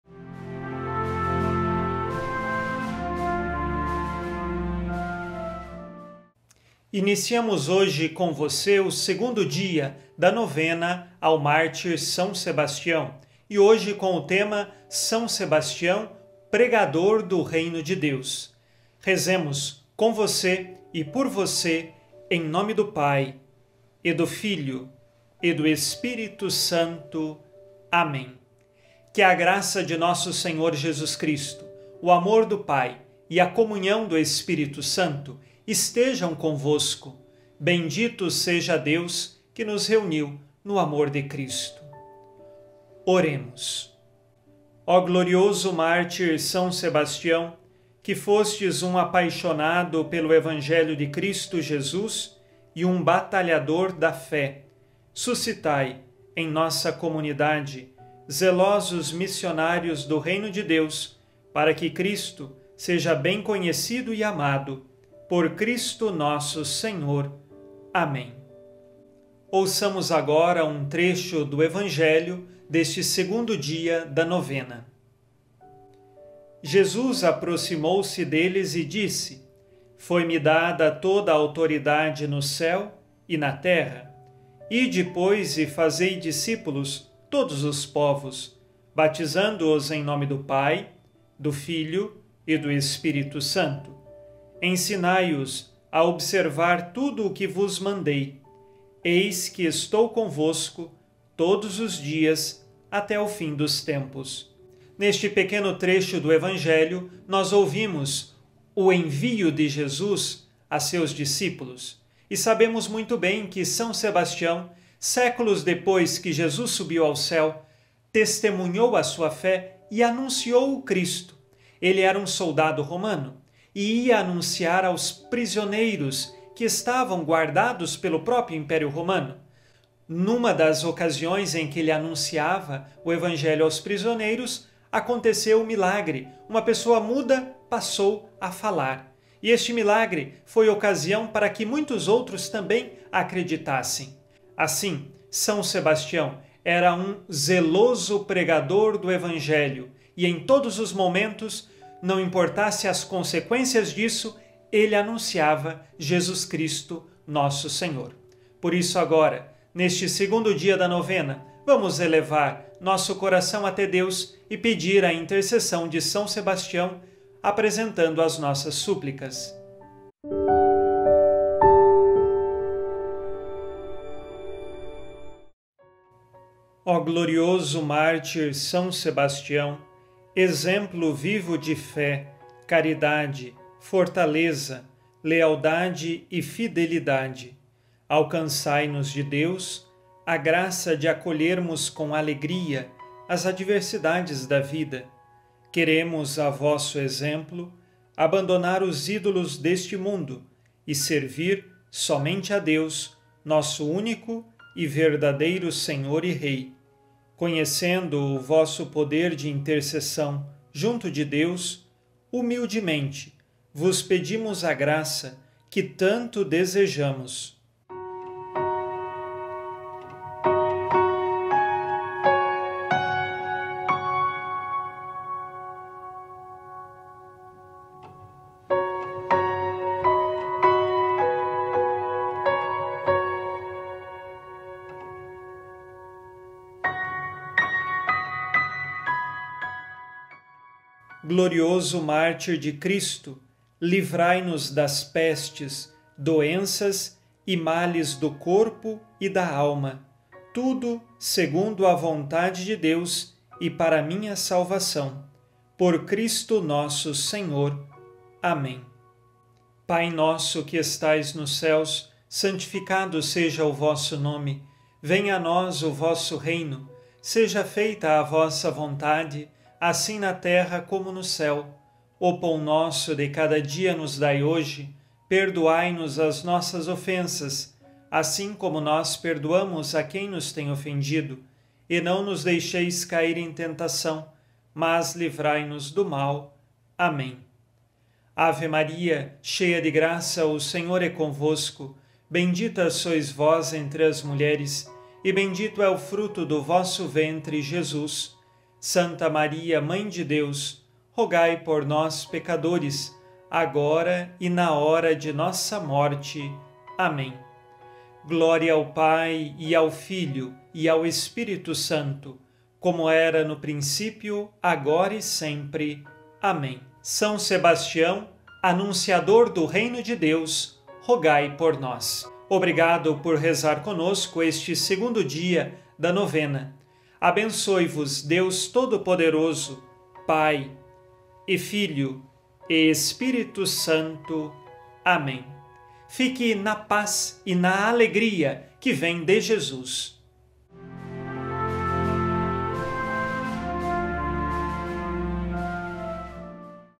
2º Dia - Novena de São Sebastião
Oração do 2º dia
Ó glorioso mártir, são Sebastião, que fostes um apaixonado pelo Evangelho de Cristo Jesus e um batalhador da fé, suscitai, em nossa comunidade, zelosos missionários do Reino de Deus, para que Cristo seja bem conhecido e amado. Por Cristo Nosso Senhor. Amém.

Oração final
Ó glorioso mártir, são Sebastião, exemplo vivo de fé, caridade, fortaleza, lealdade e fidelidade, alcançai-nos de Deus a graça de acolhermos com alegria as adversidades da vida. Queremos, a vosso exemplo, abandonar os ídolos deste mundo e servir somente a Deus, nosso único e verdadeiro Senhor e Rei. Conhecendo o vosso poder de intercessão junto de Deus, humildemente vos pedimos a graça que tanto desejamos (fazer o pedido). Glorioso mártir de Cristo, livrai-nos das pestes, doenças e males do corpo e da alma, tudo segundo a vontade de Deus e para minha salvação. Por Cristo nosso Senhor. Amém.
Pai Nosso, Ave Maria e Glória ao Pai.